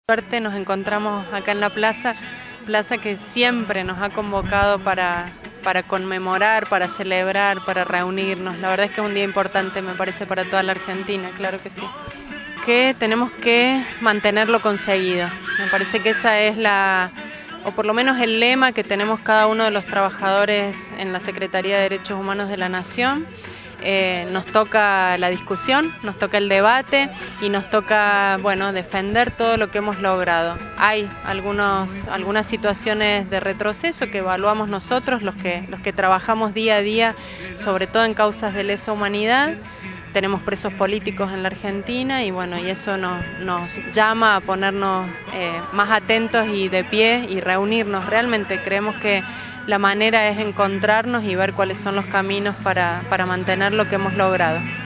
TRABAJANDO POR LOS DERECHOS HUMANOS
Gabriela Matacota de la Secretaria de Derechos Humanos de la Nación señala la importancia de trabajar por los Derechos Humanos en la Argentina